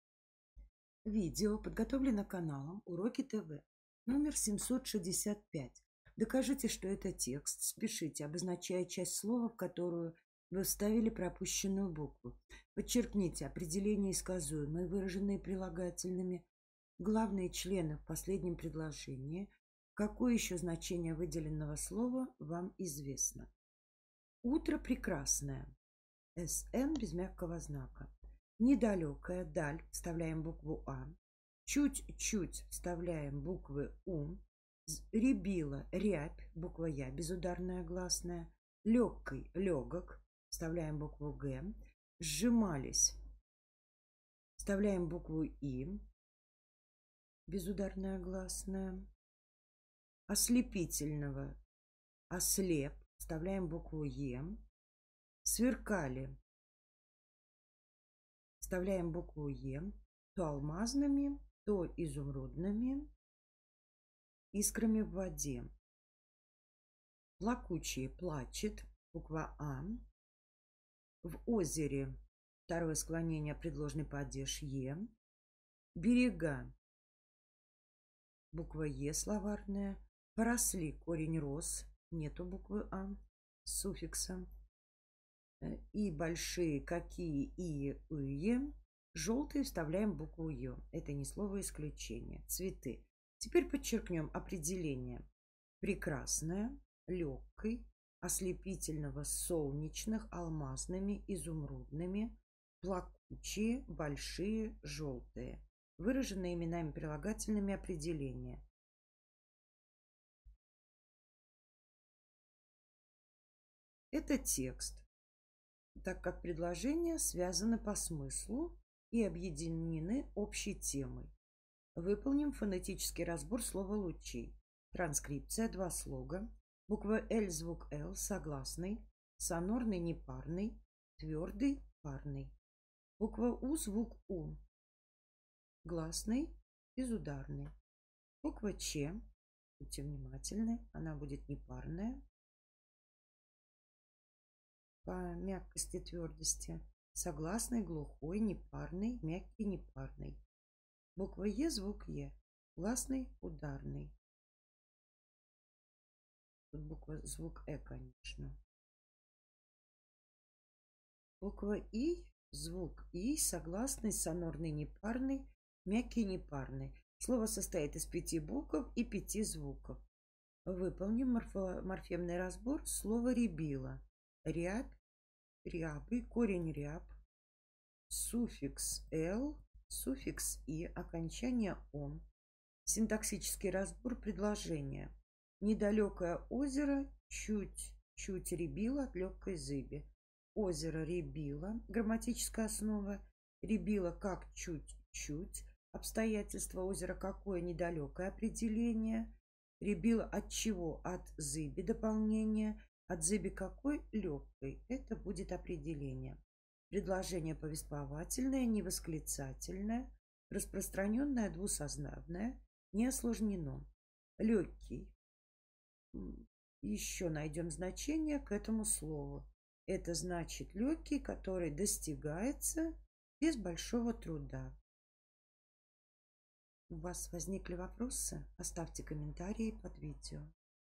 Упражнение №765 — Гдз по русскому языку 5 класс (Ладыженская) 2019 часть 2
В данном номере читаем упражнение. Доказываем, что это текст. Списываем, обозначая часть слова, в которую вставляем пропущенную букву. Подчеркиваем: 1) определения и сказуемые, выраженные прилагательными; 2) главные члены в последнем предложении. Определяем, какое ещё значение выделенного слова вам известно.